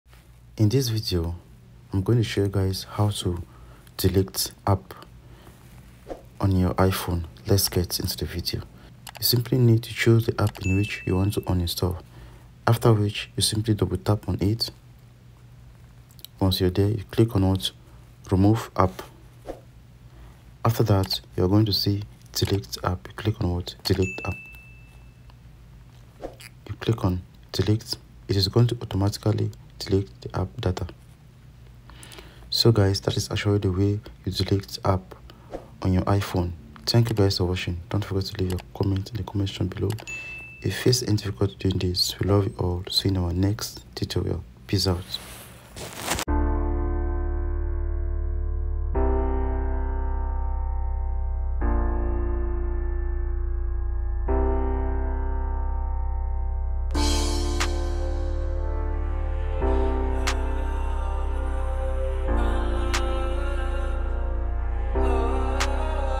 iPhone 15 / iPhone 15 Pro / iPhone 15 Pro Max: How To Uninstall / Delete Apps On iPhone - Full Guide
Learn how to easily delete apps on your iPhone 15 series with this full guide! Say goodbye to unused apps and free up storage space on your device.

With the constantly updating iPhone models, it can be confusing to figure out how to delete apps. This step-by-step tutorial will show you exactly how to delete apps on your iPhone 15 series, making managing your device a breeze. Follow along and declutter your phone in no time!

How To Delete Apps On iPhone 15 Pro max